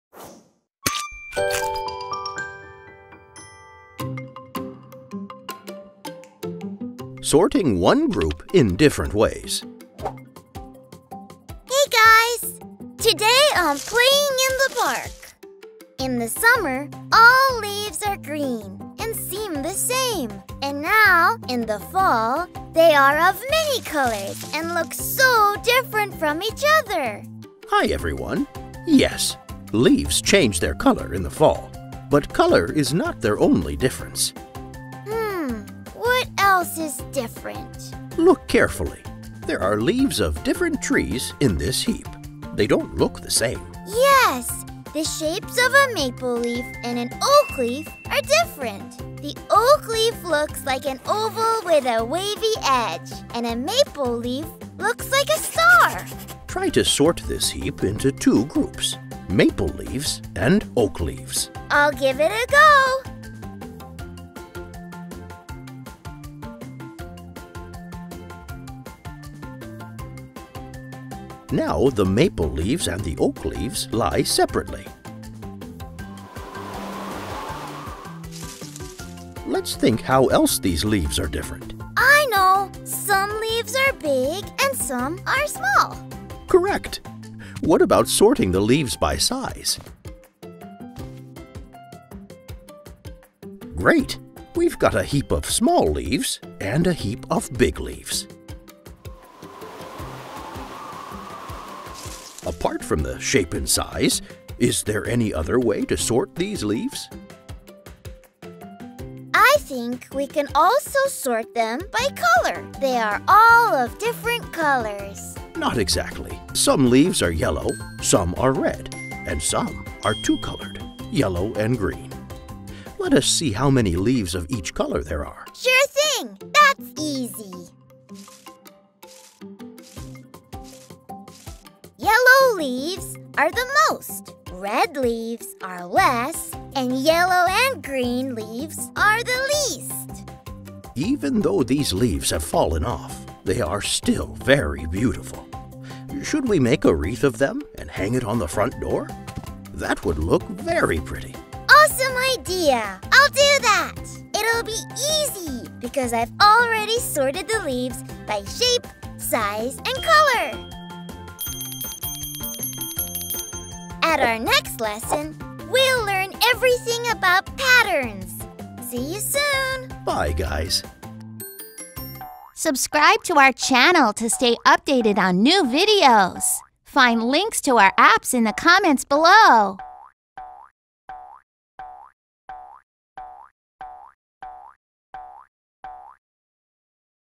Sorting One Group in Different Ways | Sorting & Matching Games for Kids | Kids Academy
Sorting objects in preschool is a necessary and vital skill. Sorting teaches and reinforces so many concepts!  Colors, shapes, sizes, describing words, vocabulary, logic, and reasoning are learned when preschool and kindergarten students sort. The simple act of sorting objects by color, size or shape prepares students for challenging problem solving as they get older.

The video depicts a scene that many students recognize. A friendly elephant is playing in some leaves in a park. The season is fall, so there are many leaves of different colors on the ground.  The elephant then sorts the leaves in different ways. The leaves are sorted by shape. Oak leaves are put in one heap and Maple leaves in another.  The leaves are different sizes as well so the leaves are sorted into piles of big and little leaves.  And finally, the leaves are sorted by color. The animation in the video is fun and inviting for young students and the language is easy to understand.

In addition to the video and digital games, hands-on sorting objects activities for preschoolers are found everywhere in the home and school environment.  Sorting toys by color, shape, and size is a fun activity. Students enjoy sorting themselves by hair color, shirt color or by who is wearing shorts or pants. Sorting pom poms, shells and even cereal are great ideas!

Once preschoolers master the basics, they are ready for more difficult activities with sorting objects in kindergarten.  Ask kindergarten students to sort objects by two attributes such as by gender and clothing. For example, sort girls wearing pants and boys wearing pants. Use this video to introduce sorting to kindergarteners or as a reteaching or introduction activity to grade-level K math.

Sorting preschool activities are an integral part of math instruction for young learners. This video engages and teaches students with bright, inviting animation and a story that allows children to “see” math in their everyday world.